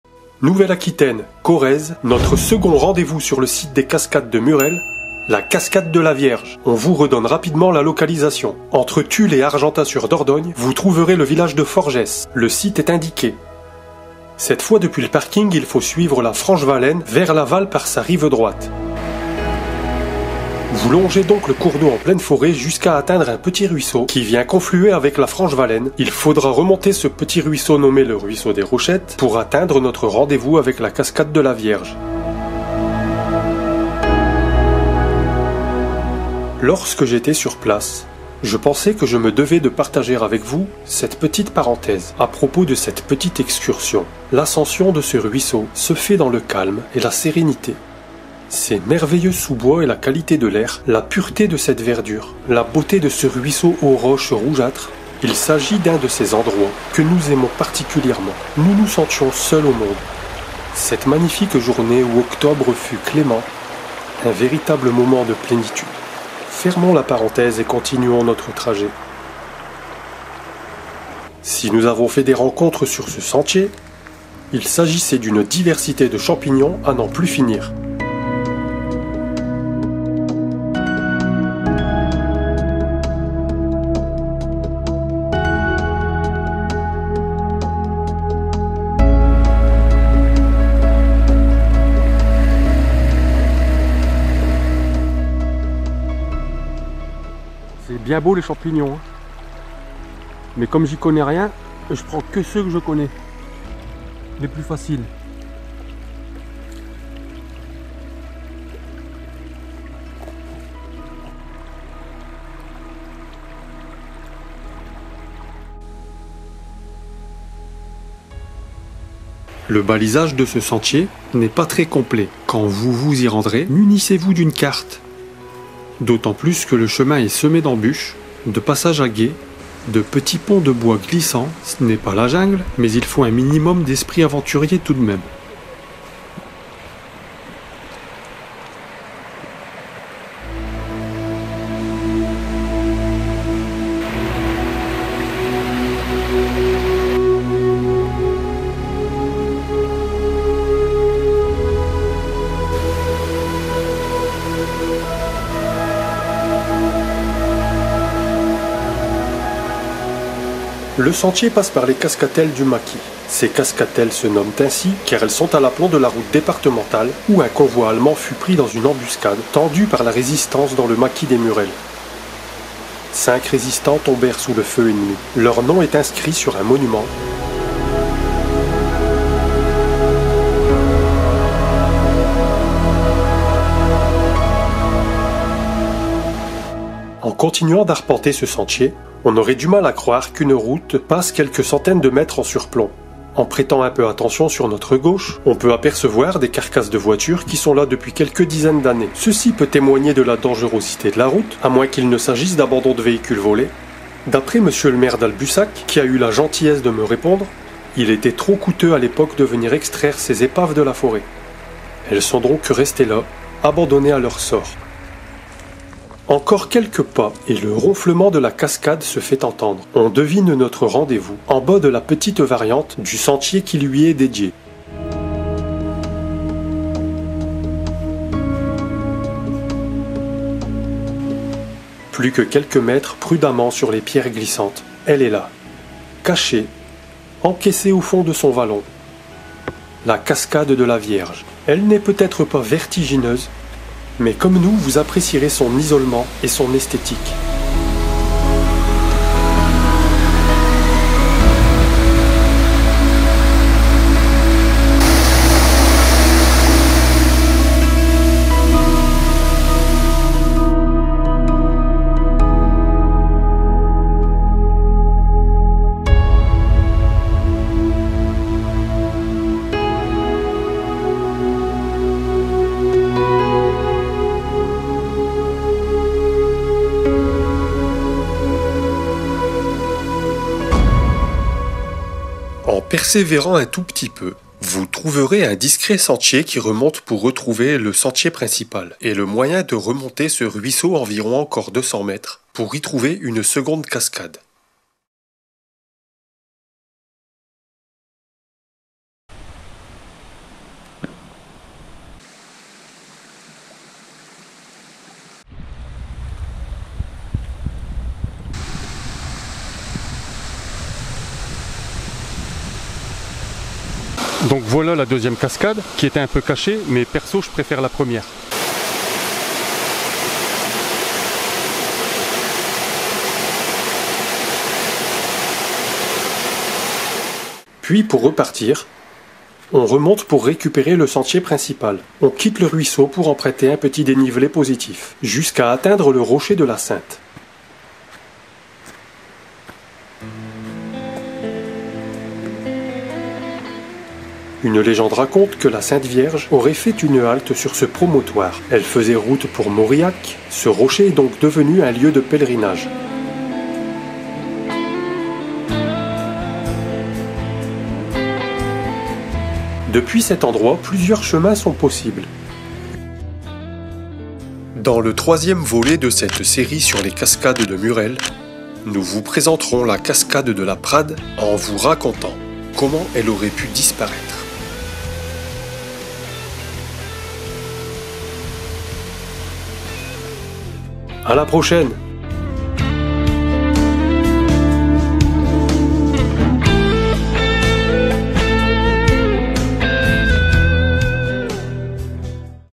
Rendez-vous: Murel 2/3 Les cascades de la Vierge
Second rendez-vous sur les site des cascades de Murel à Albussac en Corrèze, Les cascades de la Vierge

Musique de Patrick Patrikios et Thierry Bonnelle